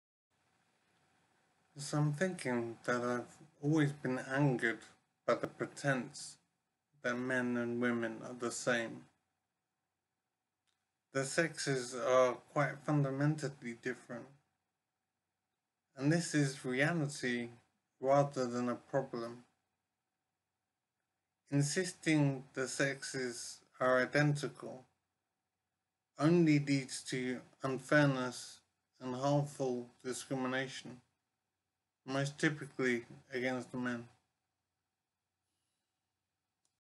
Men and women are fundamentally different!
We should stop the intense and radical insistence that the sexes are identical, because this leads to unfairness and discrimination, most typically against men.